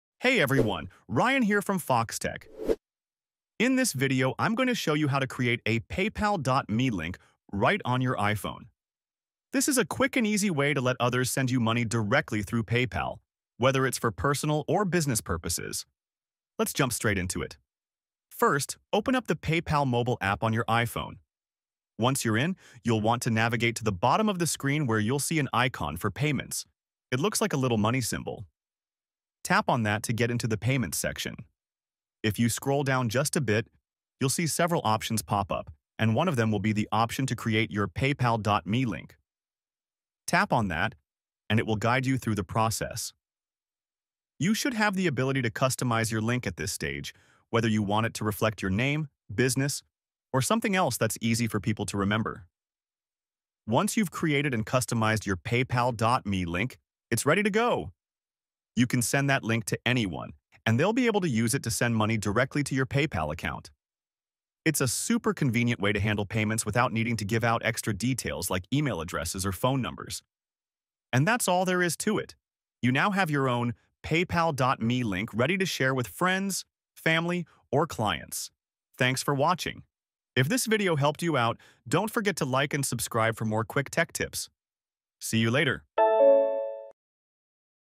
How to Create Paypal.me Link on iPhone (tutorial)
In this video, we walk you through the process of creating a [PayPal.me] link directly from your iPhone. Learn how to set up this convenient payment link to easily receive money from friends, family, or customers. Follow our simple steps to get your personalized [PayPal.me] link up and running in no time.